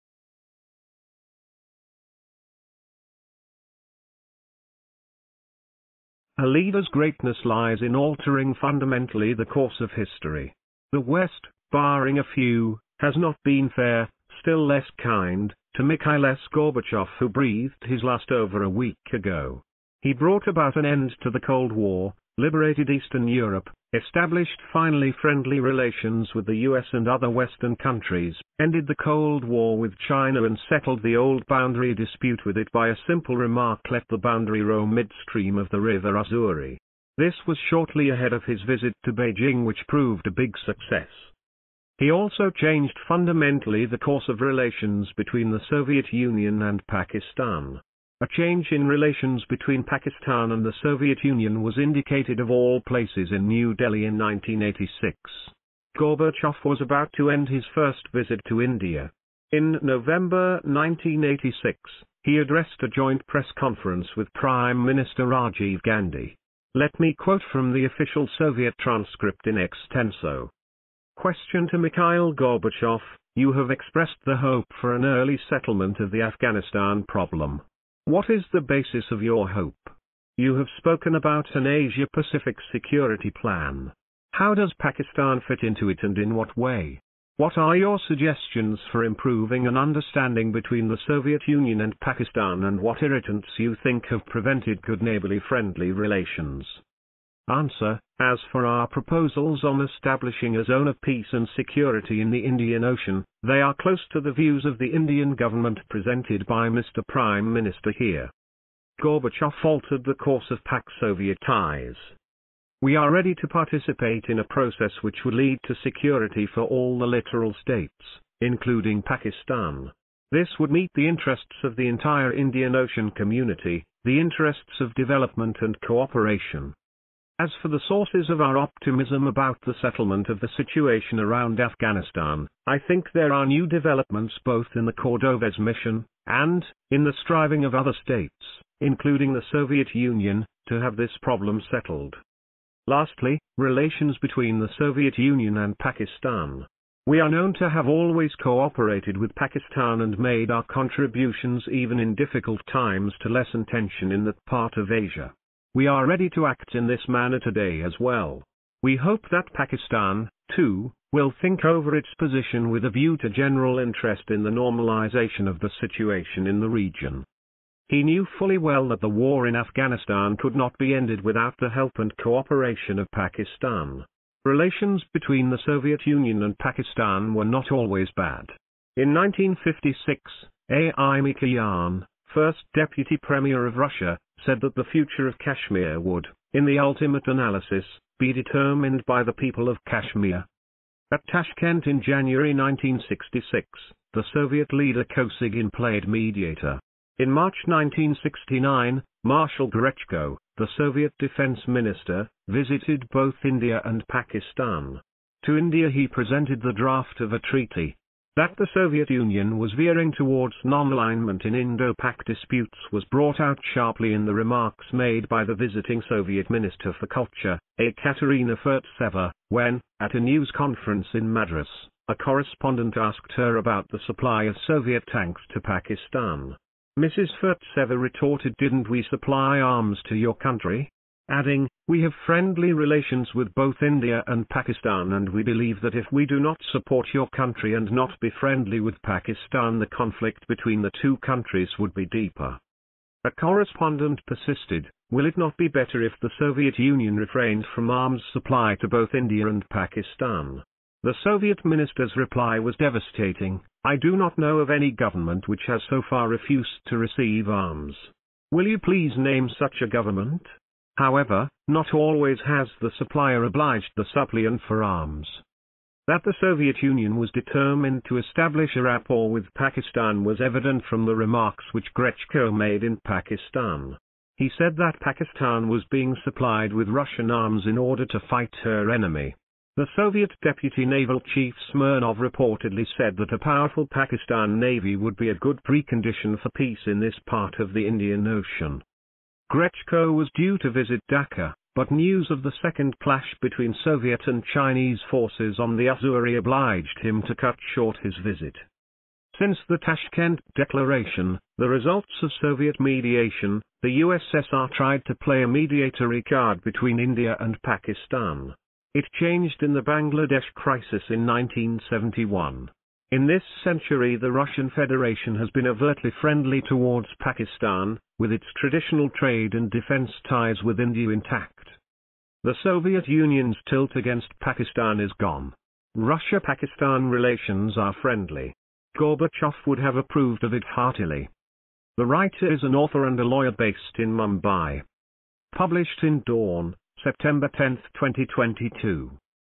Russia & S Asia
A LEADER’S greatness lies in altering fundamentally the course of history. The West, barring a few, has not been fair, still less kind, to Mikhail S. Gorbachev who brea­th­ed his last over a week ago. He brought about an end to the Cold War; liberated Eas­tern Europe, established finally friendly relations with the US and other Western countries, ended the Cold War with China and set­tled the old boundary dispute with it by a simple remark “let the boundary row midstream of the River Ussuri”. This was shortly ahead of his visit to Beijing which proved a big success.

He also changed fundamentally the course of relations between the Soviet Union and Pakistan. A change in relations between Pakistan and the Soviet Union was indicated of all places in New Delhi in 1986. Gorbachev was about to end his first visit to India. In November 1986, he addressed a joint press conference with prime minister Rajiv Gandhi. Let me quote from the official Soviet transcript in extenso.

“Question to Mikhail Gorbachev: “You have expressed the hope for an early settlement of the Afghanistan problem. What is the basis of your hope? You have spoken about an Asia-Pacific security plan. How does Pakistan fit into it and in what way? What are your suggestions for improving an understanding between the Soviet Union and Pakistan and what irritants you think have prevented good-neighbourly friendly relations?”

Answer: “As for our proposals on establishing a zone of peace and security in the Indian Ocean, they are close to the views of the Indian Government presented by Mr Prime Minister here.

Gorbachev altered the course of Pak-Soviet ties.

“We are ready to participate in a process which would lead to security for all the littoral states, including Pakistan. This would meet the interests of the entire Indian Ocean community, the interests of development and cooperation.

“As for the sources of our optimism about the settlement of the situation around Afghanistan, I think there are new developments both in the Cordovez Mission [and] in the striving of other states, including the Soviet Union, to have this problem settled. …

“Lastly, relations between the Soviet Union and Pakistan. We are known to have always cooperated with Pakistan and made our contributions even in difficult times to lessen tension in that part of Asia. We are ready to act in this manner today as well. We hope that Pakistan, too, will think over its position with a view to general interest in the normalisation of the situation in the region.”

He knew fully well that the war in Afghanistan could not be ended without the help and cooperation of Pakistan. Relations between the Soviet Union and Pakistan were not always bad. In 1956, A.I. Mikoyan, first deputy premier of Russia, said that the future of Kashmir would, in the ultimate analysis, be determined by the people of Kashmir.

At Tashkent in January 1966, the Soviet leader Kosygin played mediator. In March 1969, Marshal Grechko, the Soviet defence minister, visited both India and Pakistan. To India he presented the draft of a treaty. That the Soviet Union was veering towards non-alignment in Indo-Pak disputes was brought out sharply in the remarks made by the visiting Soviet minister for culture, Ekaterina Furtseva, when, at a news conference in Mad­ras, a correspondent asked her about the supply of Soviet tanks to Pakistan. Mrs Furtseva retorted “Didn’t we supply arms to your country?”, adding, “We have friendly relations with both India and Pakistan and we believe that if we do not support your country and not be friendly with Pakistan the conflict between the two countries would be deeper.”

A correspondent persisted: “Will it not be better if the Soviet Union refrained from arms supply to both India and Pakistan”. The Soviet minister’s reply was devastating: “I do not know of any government which has so far refused to receive arms. Will you please name such a government?” However, not always has the supplier obliged the suppliant for arms.

That the Soviet Union was determined to establish a rapport with Pakistan was evident from the remarks which Grechko made in Pakistan. He said that Pakistan was being supplied with Russian arms in order to fight her “enemy”. The Soviet deputy naval chief Smirnov reportedly said that “a powerful Pakistan Navy would be a good precondition for peace in this part of the Indian Ocean.”